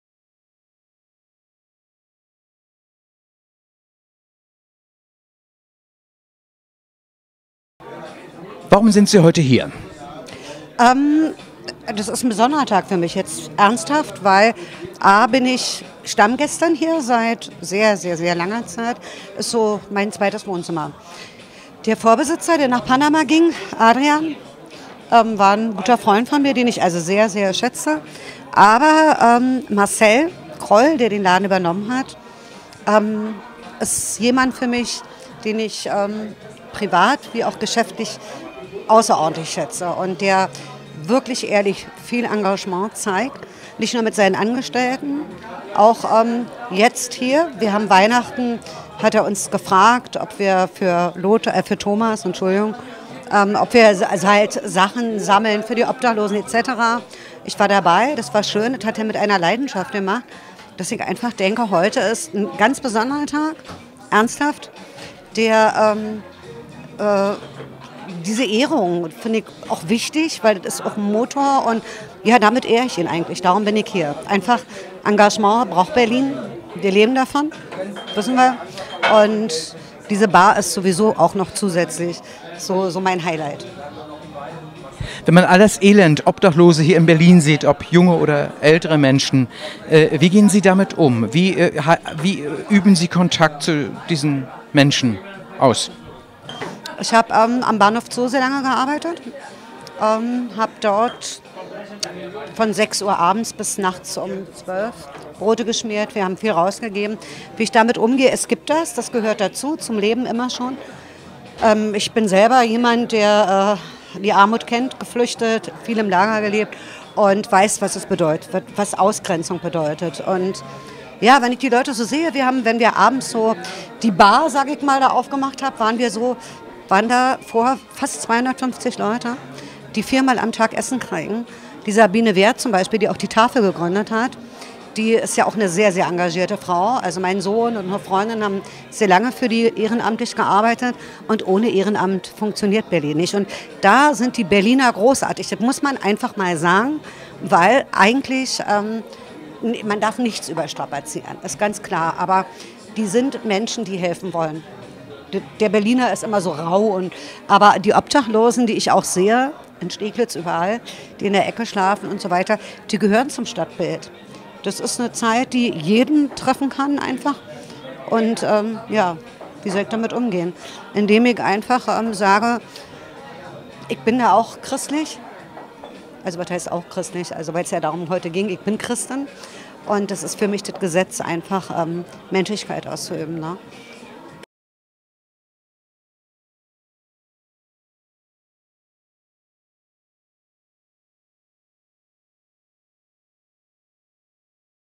Interview mit Heidelinde Auer
Interview mit Heidelinde Auer zum Thema  Armut und soziales  Engagement